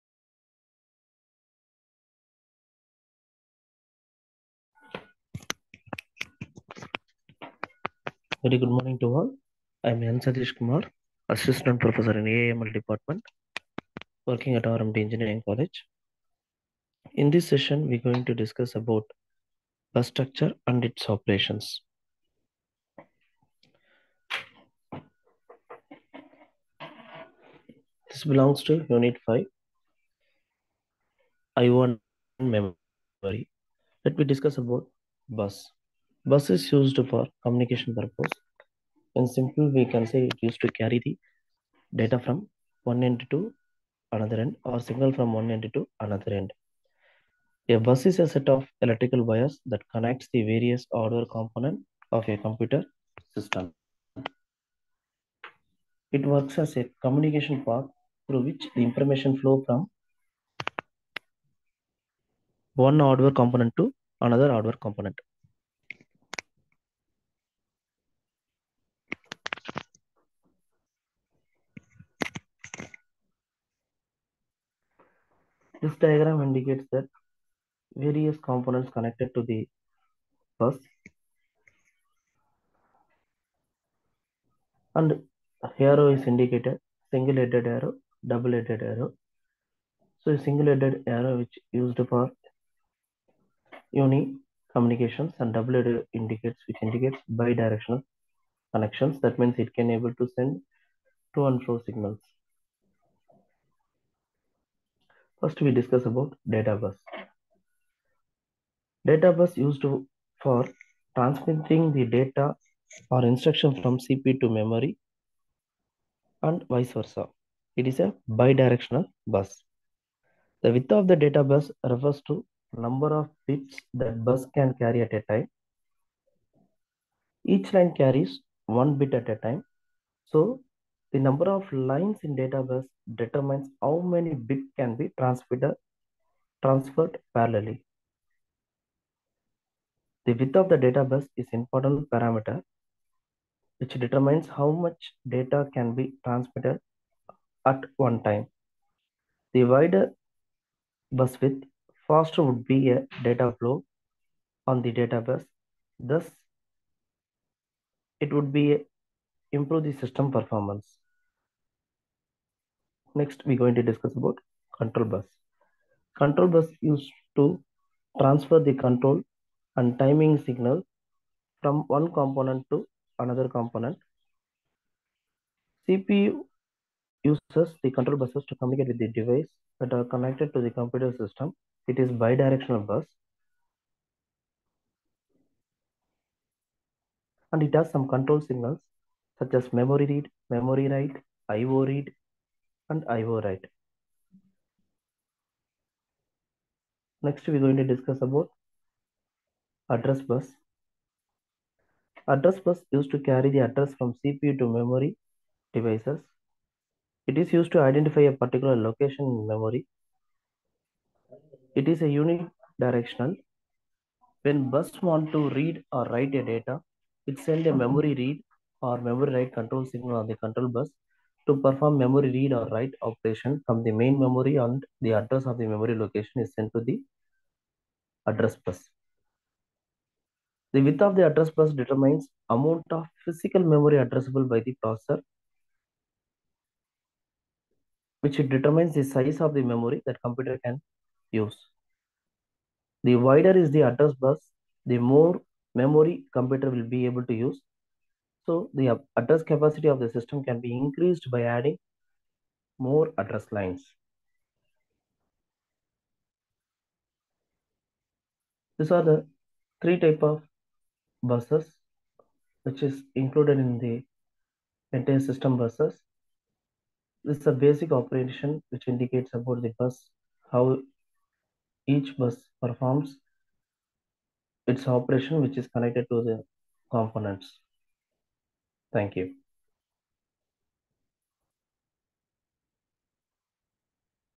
BUS- Mr. Sathish Kumar, Assistant Professor,/AIML,RMDEC
Explanation on BUS and the architecture